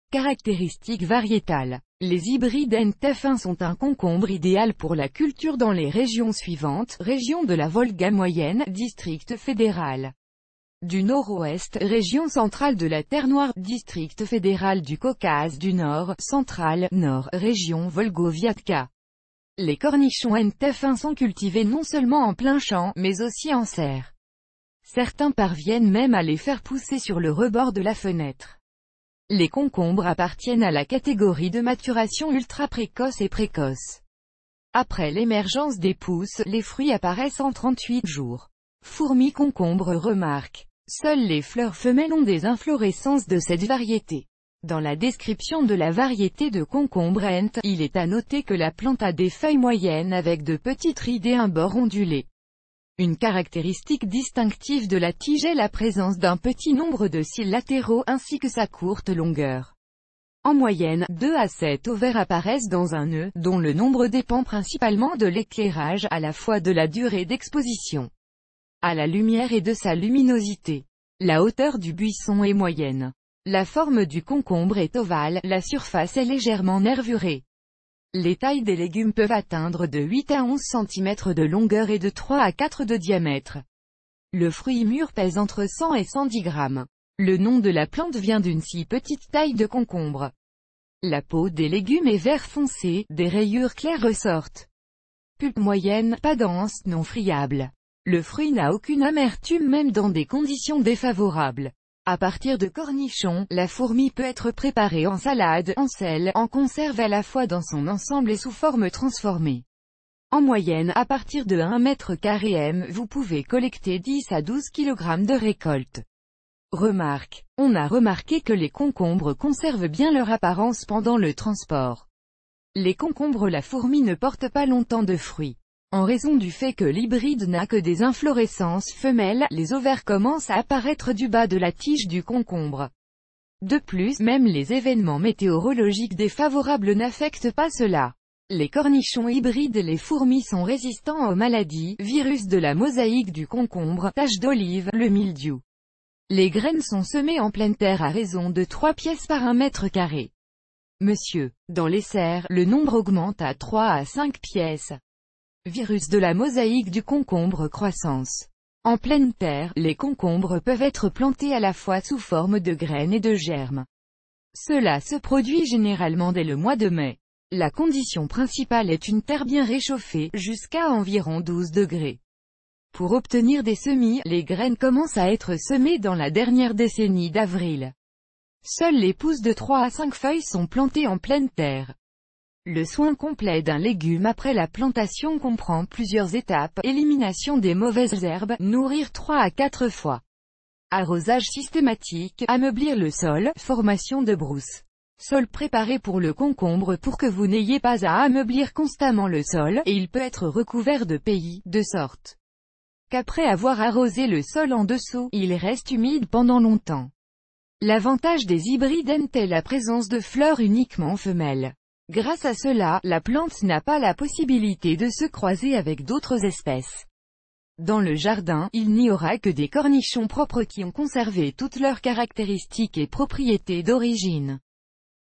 Fourmi concombre
Cucumber Ant F1: description de la variété, caractéristiques, comment planter correctement, comment les entretenir, la technologie agricole de culture, le rendement. Ce que vous devez savoir sur cette variété, des conseils et des recommandations de résidents d'été et de jardiniers expérimentés.